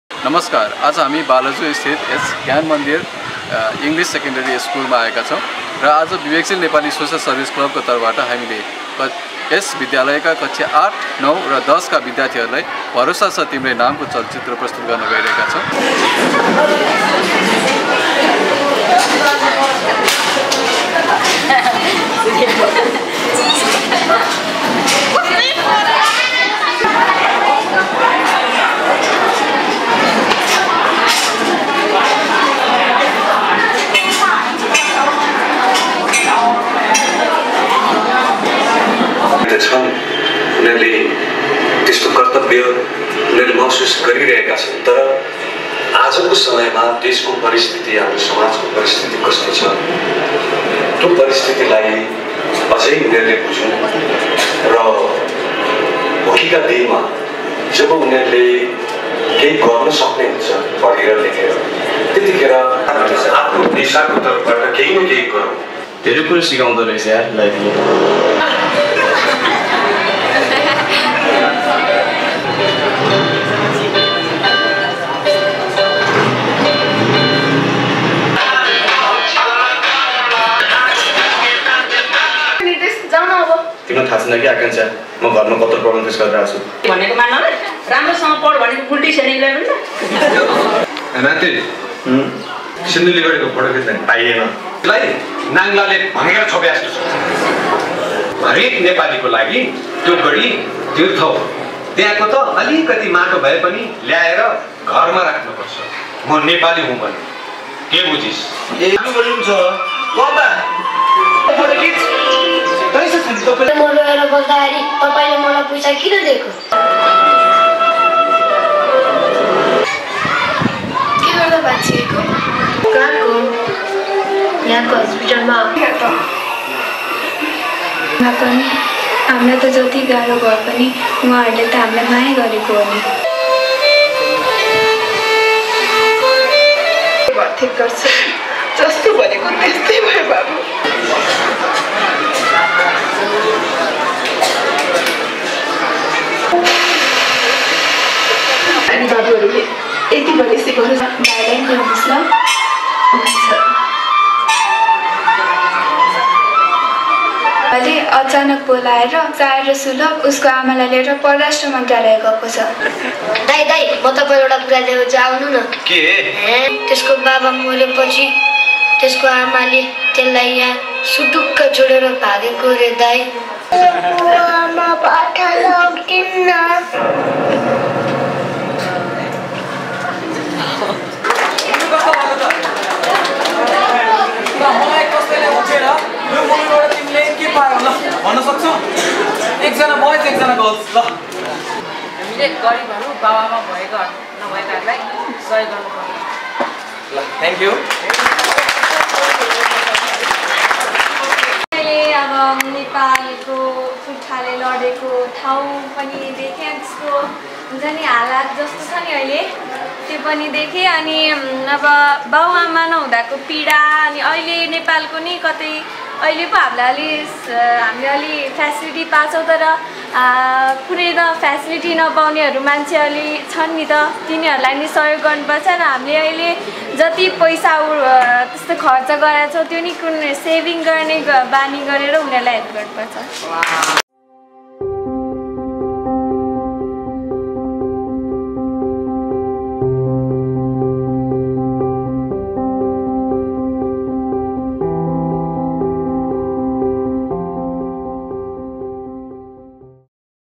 विवेकशील नेतृत्वहरु विवेकशील नागरिक निर्माणमा जुट्दै
नागरिकहरु जिम्मेवार भए मात्रै देश अगाडी बढ्छ l त्यसैले नागरिकहरुलाई विवेकशील बनाउन विवेकशील नेपाली दल समाज सेवा क्लबको पहलमा "भरोसा छ तिम्रै" लघु चलचित्र मार्फत बालाजु स्थित ज्ञान मन्दिर अंग्रेजी माध्यमिक विद्यालयका विद्यार्थीहरुलाई देश प्रति जिम्मेवार बन्न प्रोत्साहन गर्दै l